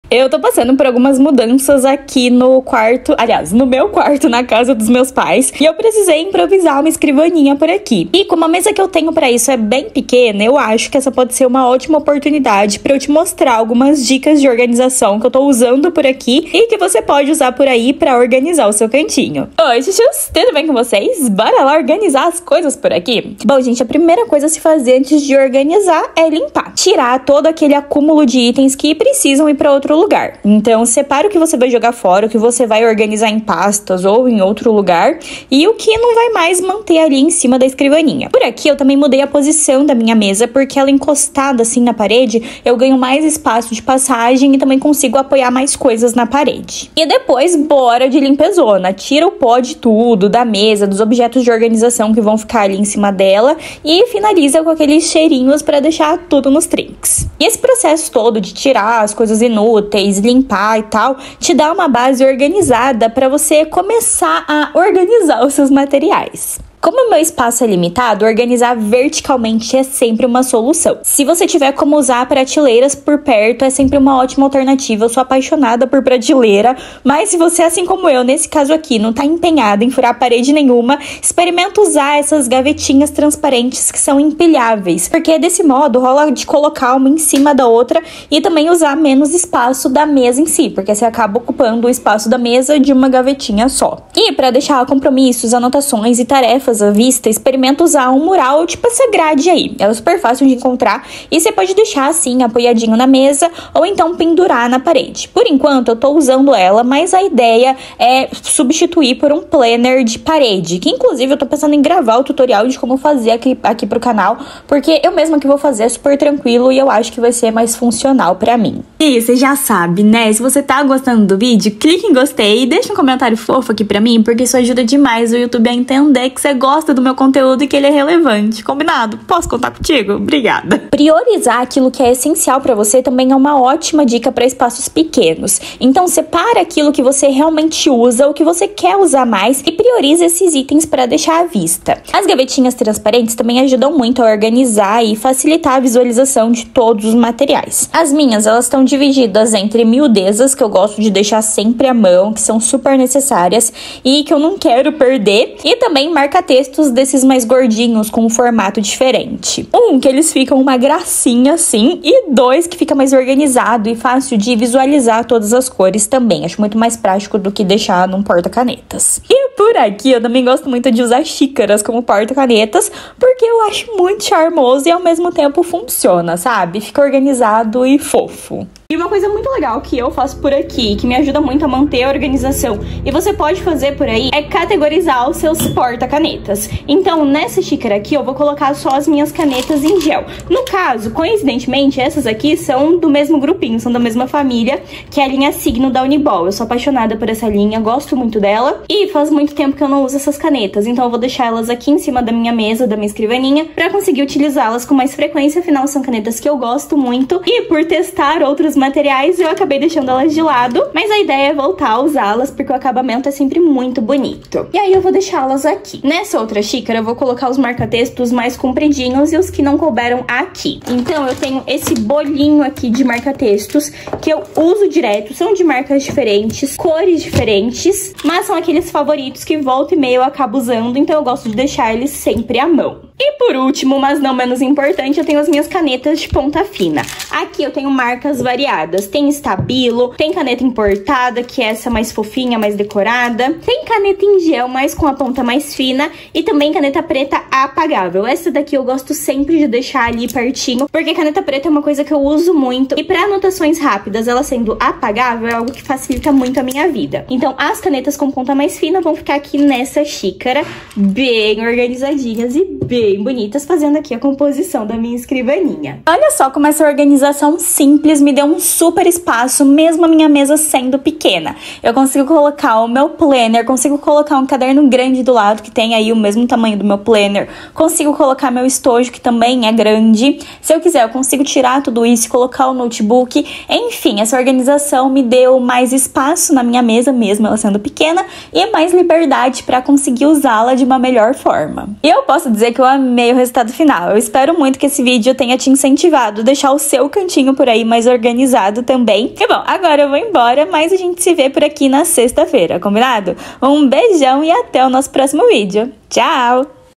COMO ORGANIZAR UMA ESCRIVANINHA PEQUENA - Karina Idalgo ♥
Nesse vídeo aqui eu te mostro como organizei minha escrivaninha improvisada, que é beeem pequena, e transformei esse espaço pra ficar confortável e super bacana pra acomodar meus materiais de papelaria!

E eu tbm tô por aqui, ó:
Meu Ig: @karinaidalgoo
Ig da minha Papelaria: @inspirapapelaria

Beeeeeeijos ♥